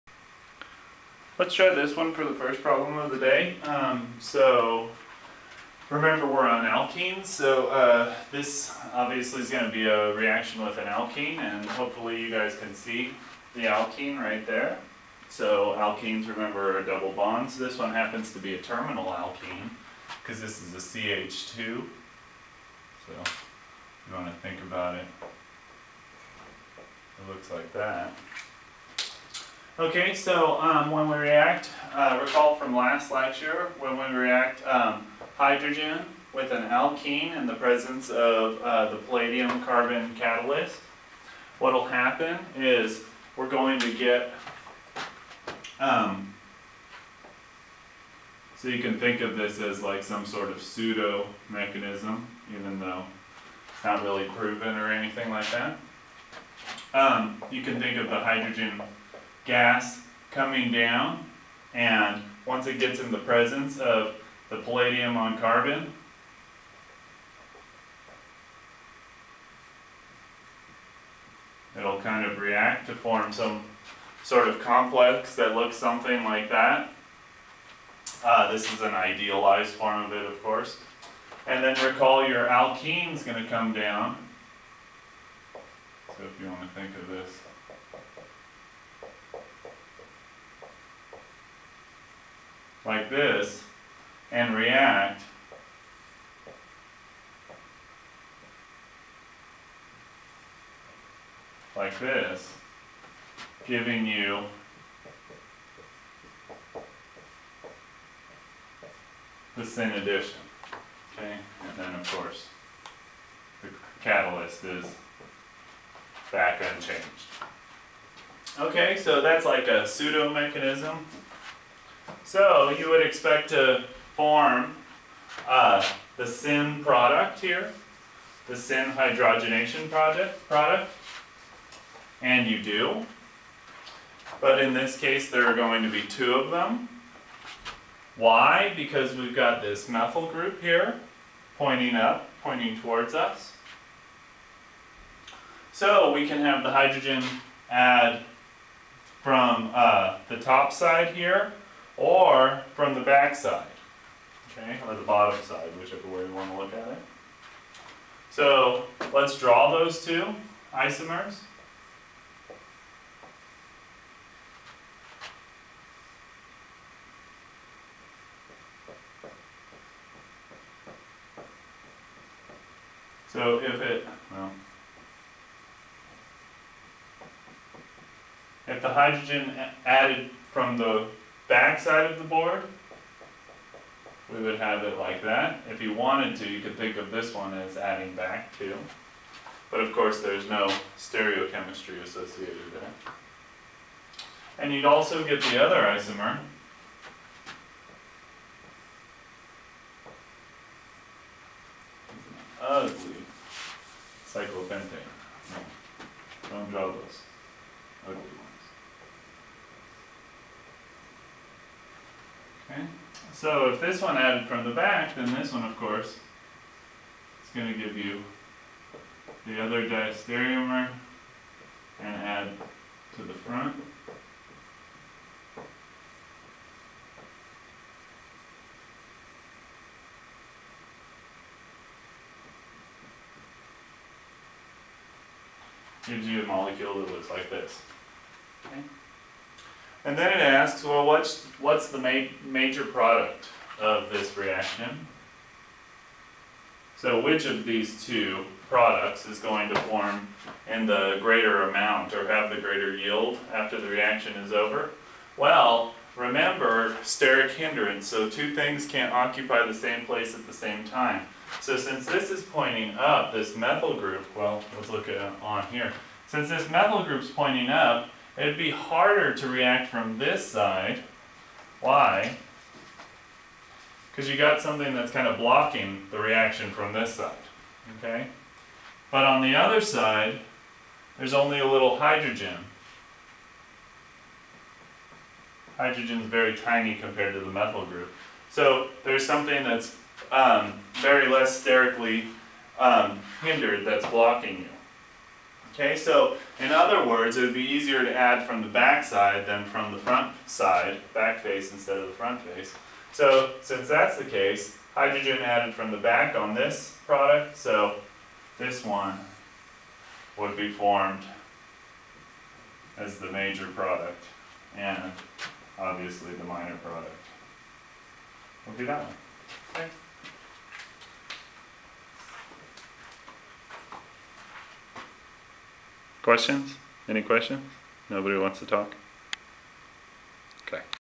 Hydrogenation of an Alkene 001
What stereoisomers are formed in the hydrogenation of (S)-2-methyl methylenecyclopentane with a palladium on carbon catalyst? Identify the major product?